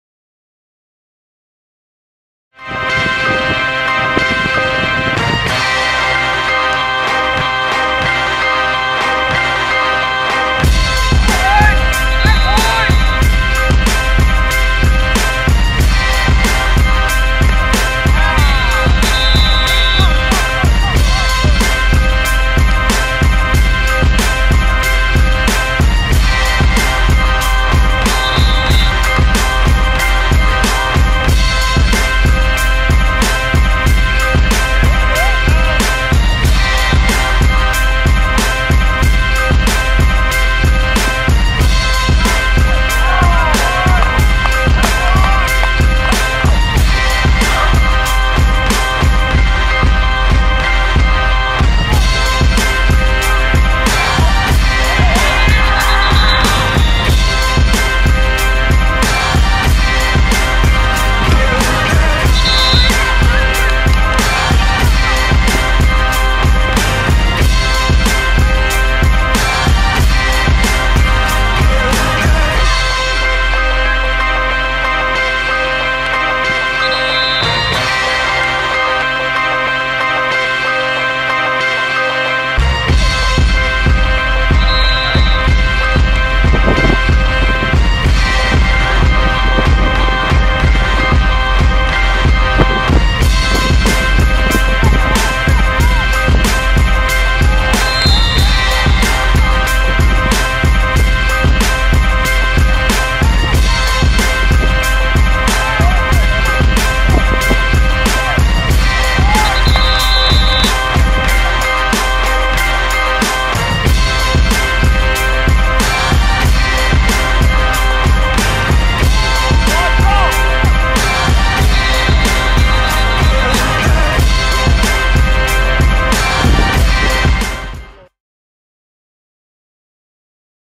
St Anthony Football Intrasquad Scrimmage
We've got highlights from the St. Anthony intrasquad scrimmage on Saturday.